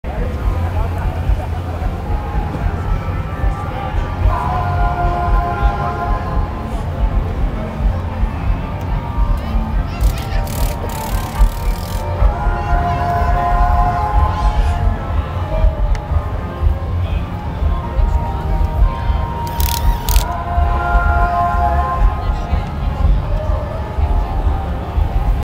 Bonnaroo Tear Drop Art
Each cage is covered with little tiles that people can paint anyway they want.  The guys working there said that they were going to be put up again next year.